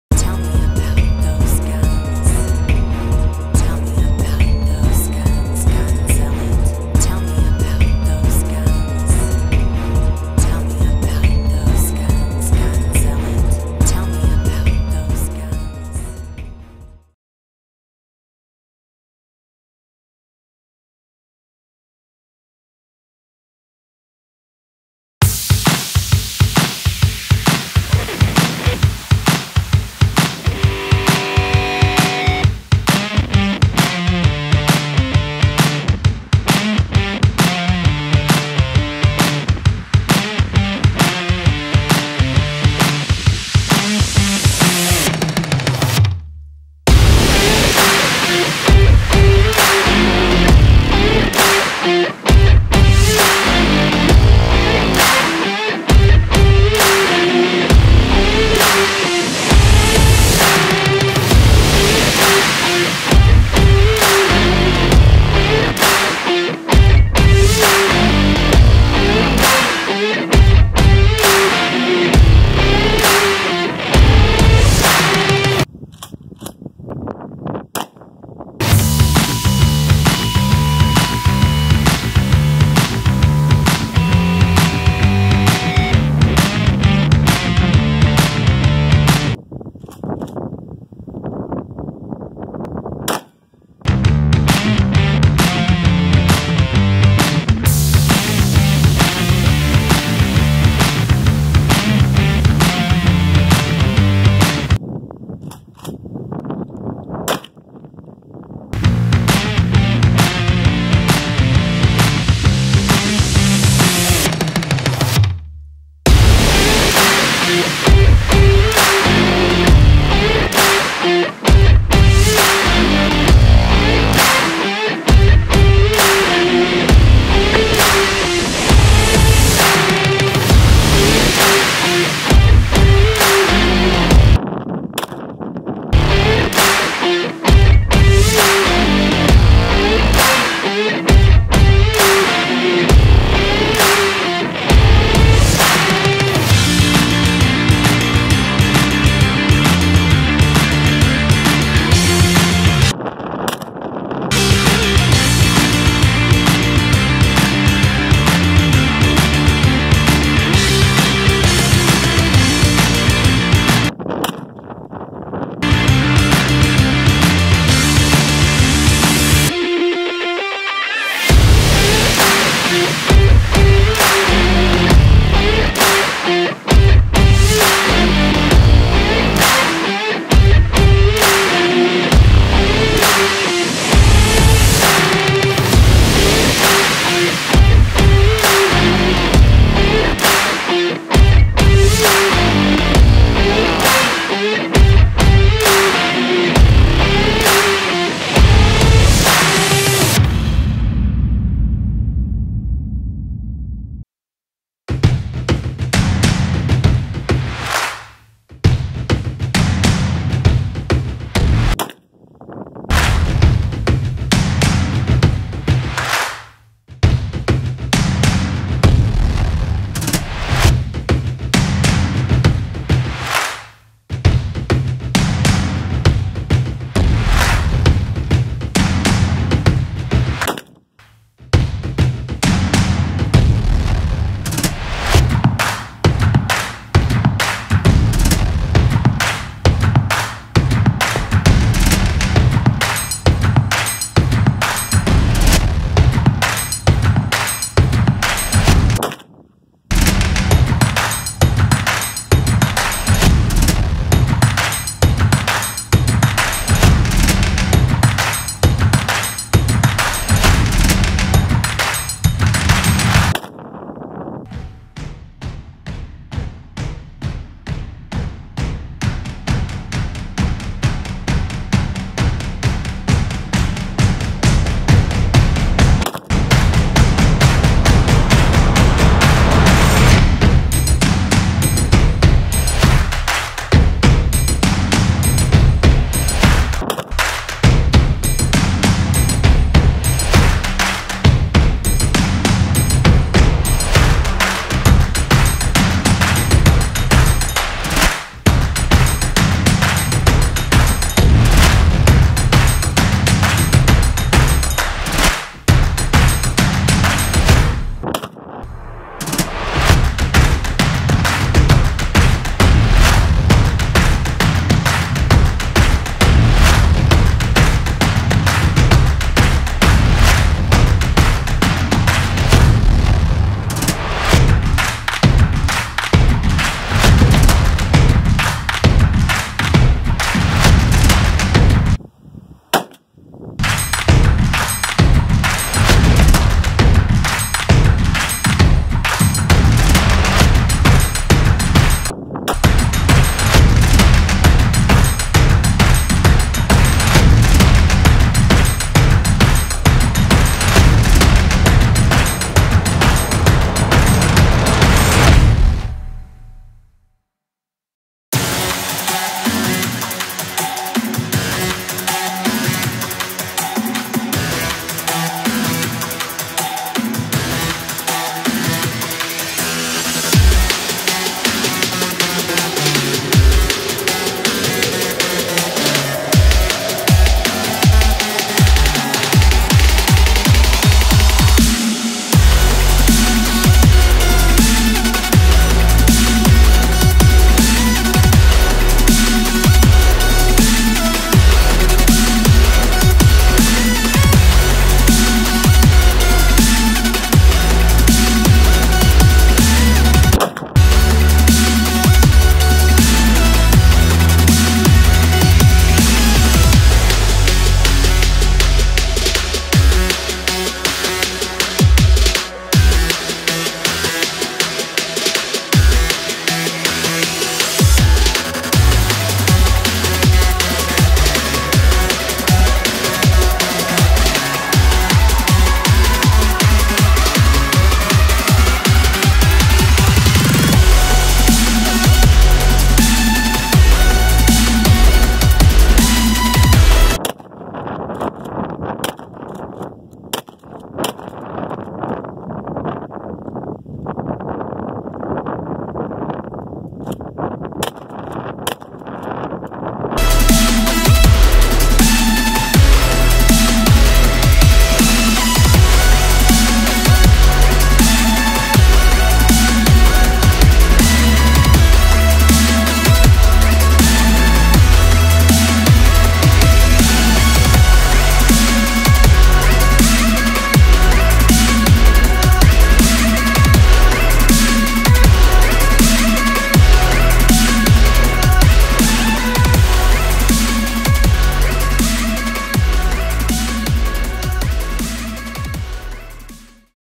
Remington 870 Tactical! HERE COMES THE BOOM!!💥 💥
Non talking review of the Remington 870 Tactical. Music only. I really enjoy this gun. It has many applications. Main one being home defense. and ground squirrel control.